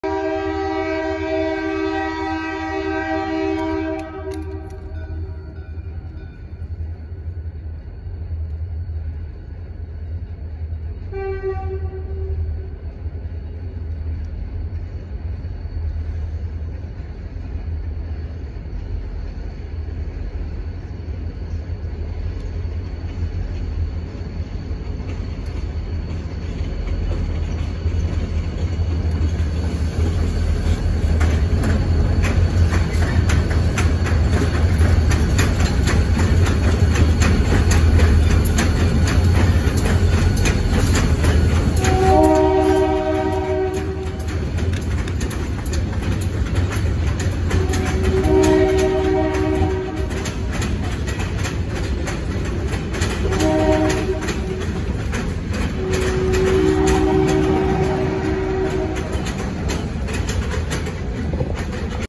Pan-Am RUPO with FOUR GP40-2LW’s crossing the Dead River at North Leeds Maine
Pan-Am RUPO/CSX L054 westbound down the Rumford Branch crossing the Dead River with four matched blue PAR GP40-2LW’s 09-28-23